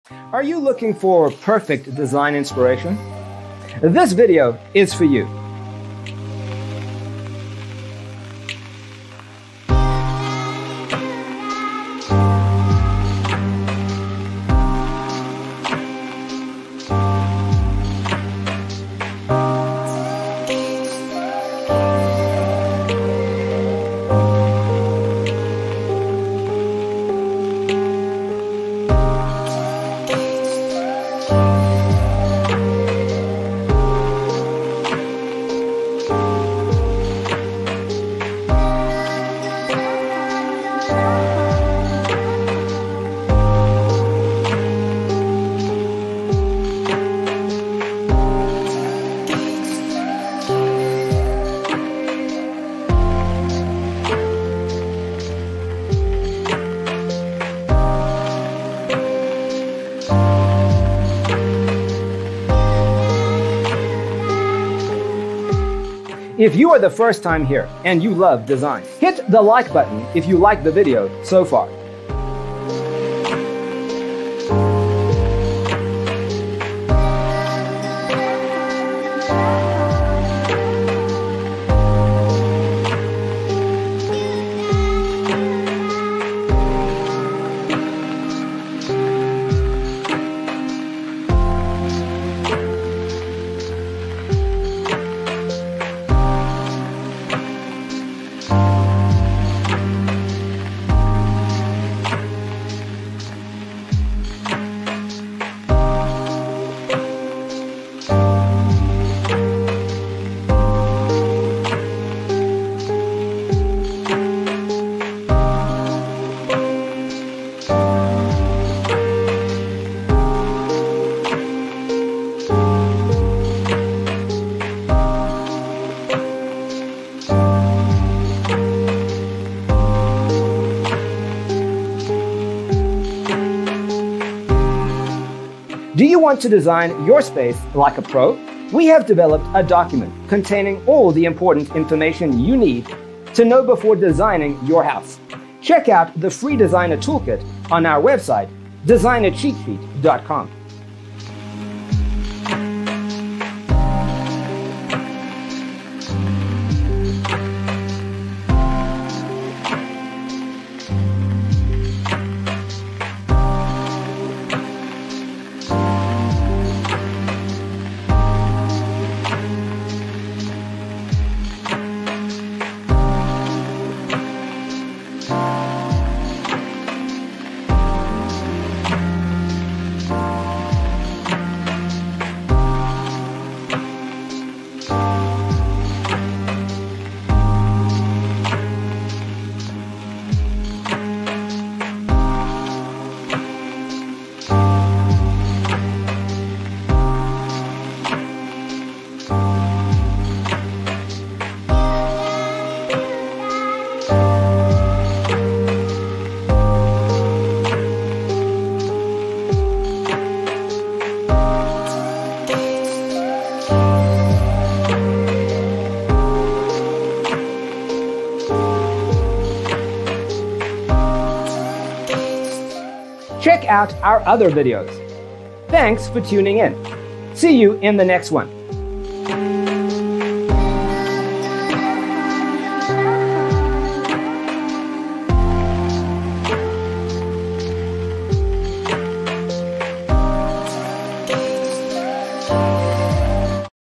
75 Small Slate Floor Family Room Design Ideas You'll Love 🔴
0:00:00 - Intro
0:00:12 - Small Slate Floor Family Room Design Ideas You'll Love: Inspirations 1 to 25
0:01:16 - Small Slate Floor Family Room Design Ideas You'll Love: Inspirations 26 to 50
0:02:31 - Small Slate Floor Family Room Design Ideas You'll Love: Inspirations 51 to 75